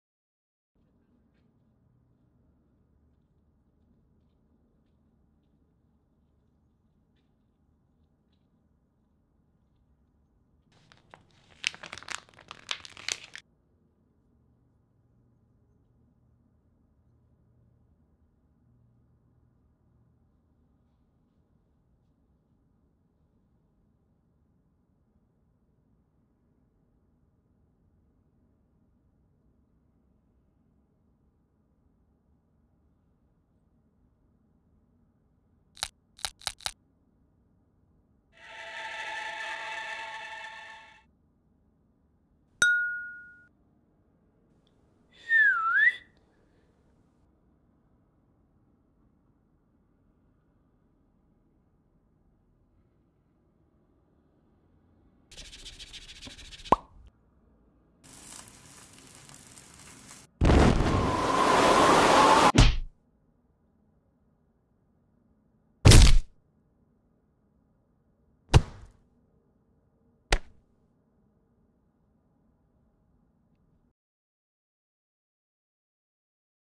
Animated Short Film | R.I.P
A stop motion film a made a while ago that got shown at the BFI. I'm still trying to get better at stop motion, particularly my sound design. But practice makes perfect